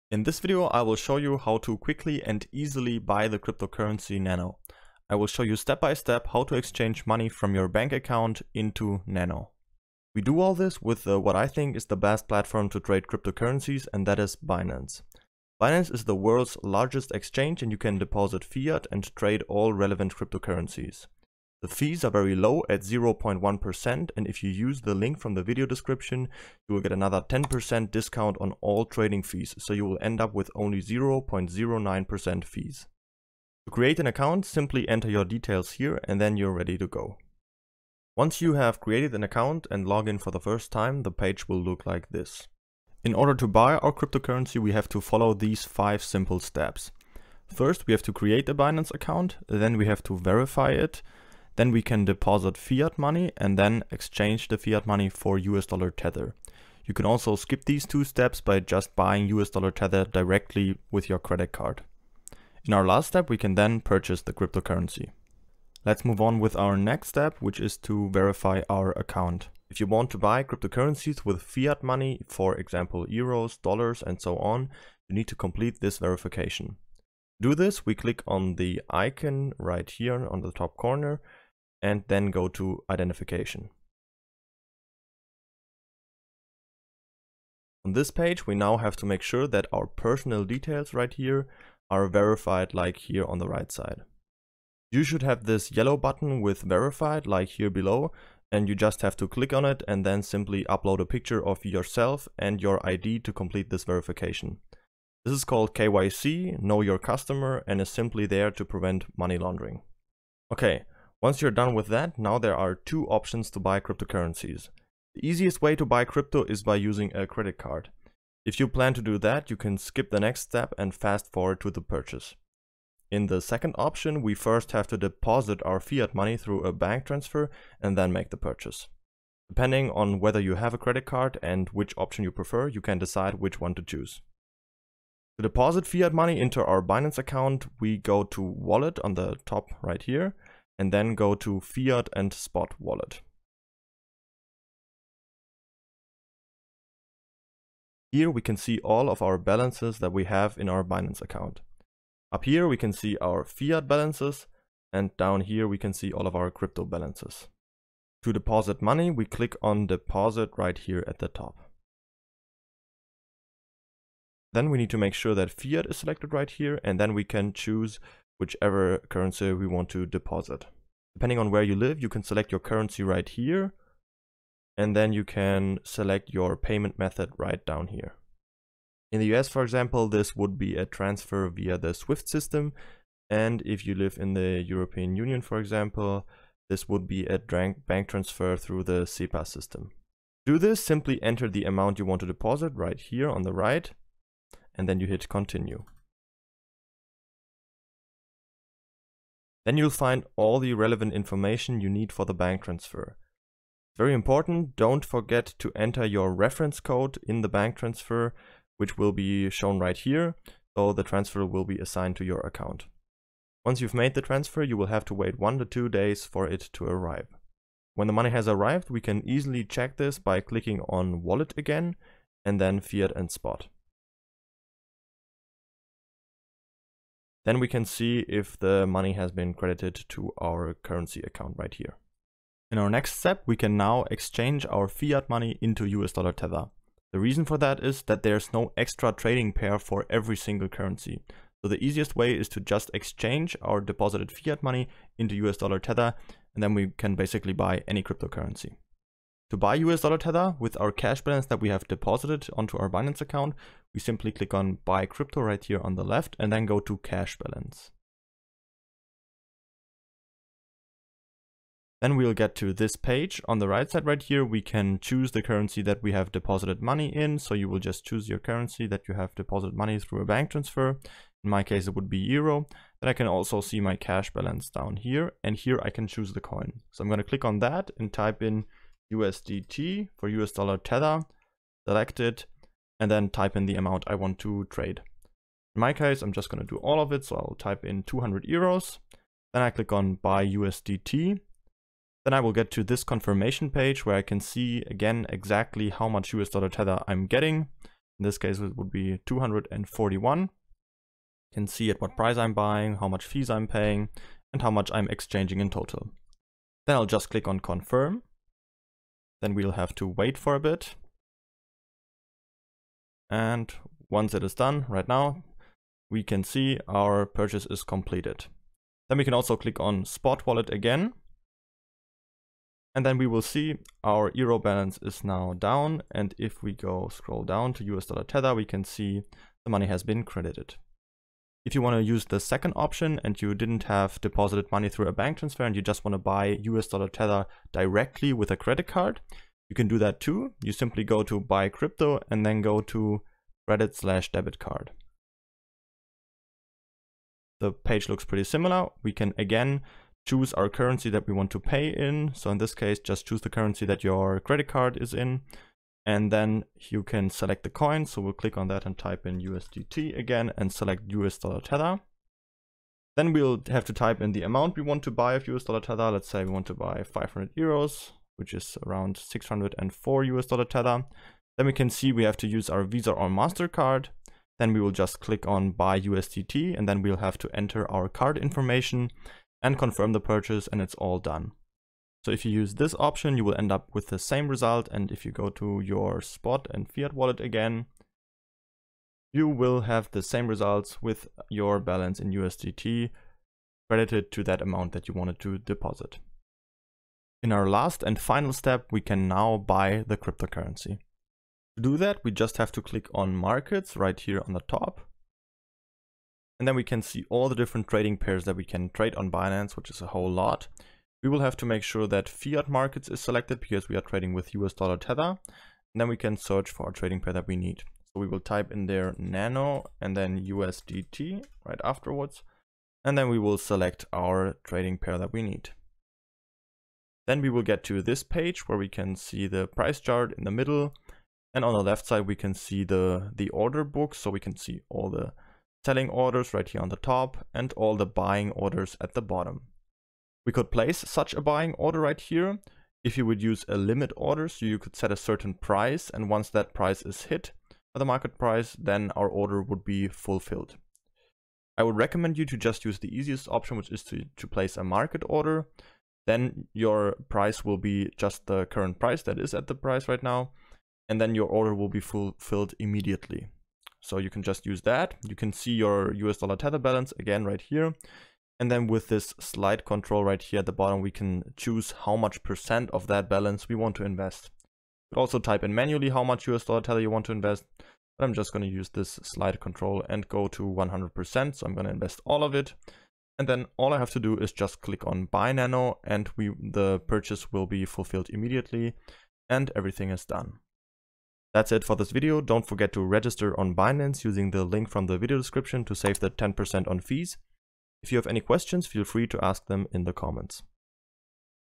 How to buy NANO ✅ Step-by-Step Tutorial
This video will explain you how to buy NANO quickly and easily in a few steps.

⏱️Timestamps⏱️
00:00 Introduction
00:10 Step 1: Create a Binance Account
01:06 Step 2: Verification
02:15 Step 3: Deposit money via bank transfer
03:53 Step 4: Exchange Fiat for USD Tether
05:31 Alternative: Buy USD Tether directly by credit card
06:37 Buy NANO
08:15 End